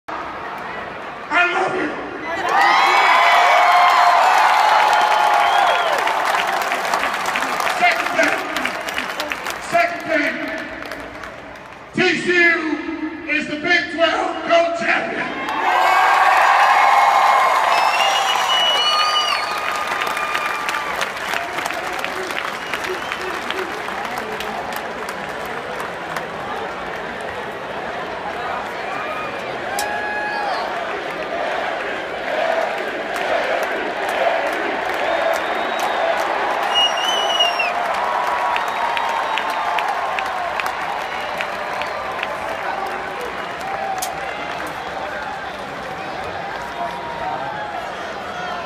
TCU's Gary Patterson accepts Big 12 trophy
The coach addresses fans after the Horned Frogs 55-3 victory over Iowa State in Fort Worth.